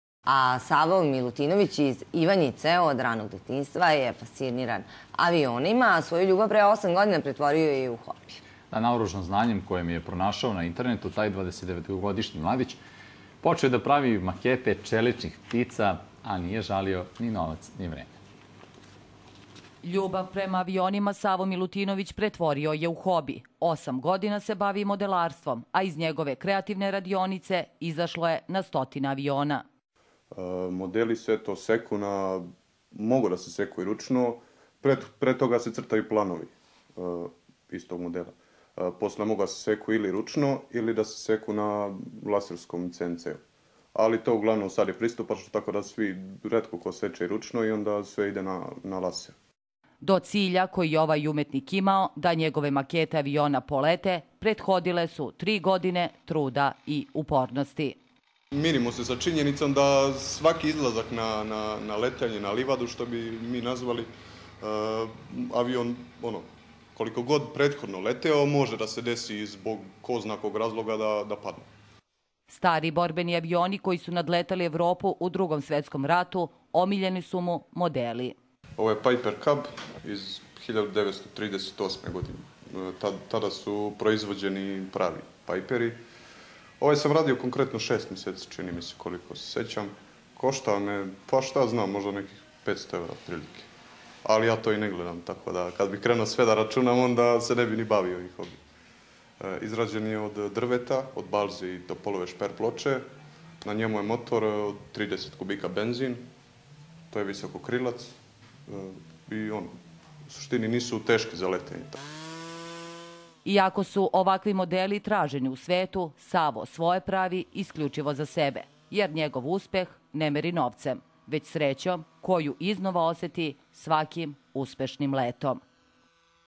Savo Milutinović iz Ivanjice pravi makete čeličnih ptica
Savo Milutinović iz Ivanjice od ranog detinjstva fasciniran je avionima, a svoju ljubav pre osam godina pretvorio je u hobi. Naoružan znanjem koje je pronašao na internetu, taj  29-оgodišnji  mladić počeo je da pravi makete čeličnih ptica, a nije žalio ni novac i vreme.